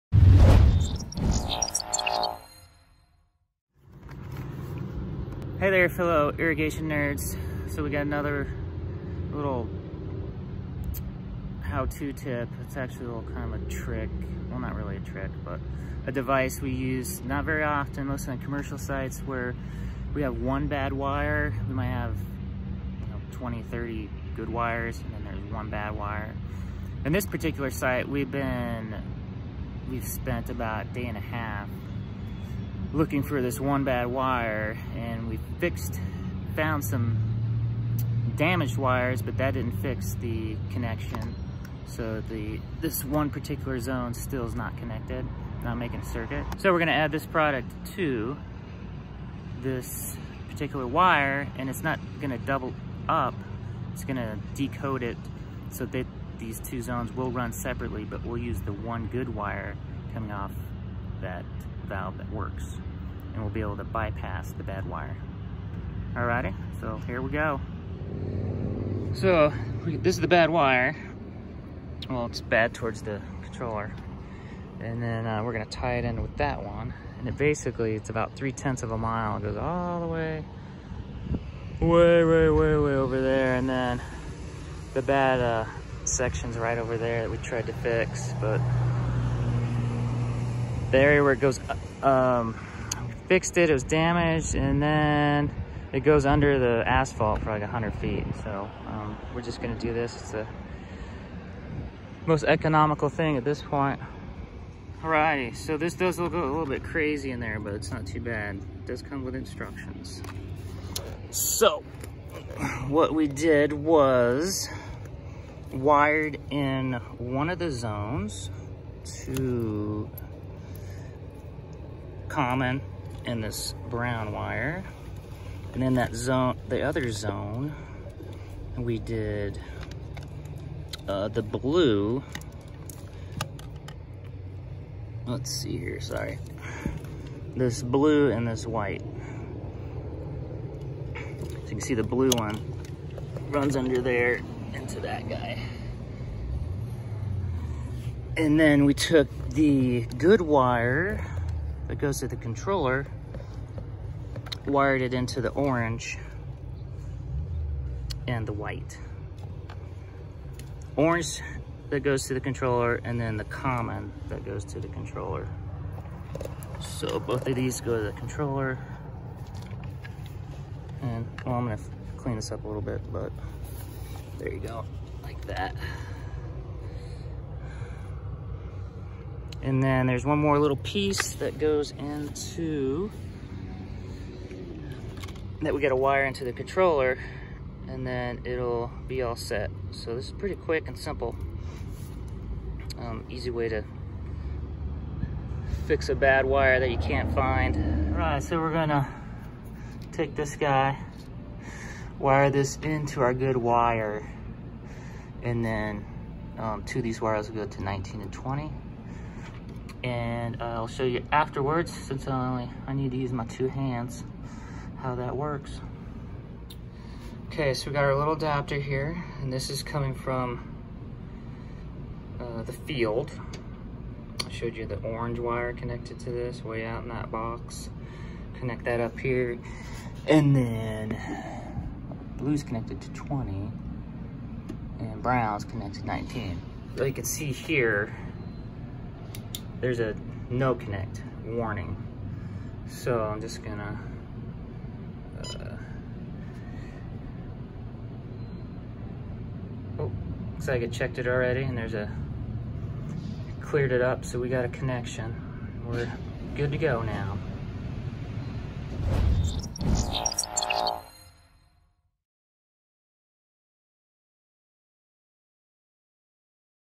Easy way to fix or add a wire for irrigation zone
Use a product to add a zone and not have to dig a new trench and install new wire.

Flagstaff, AZ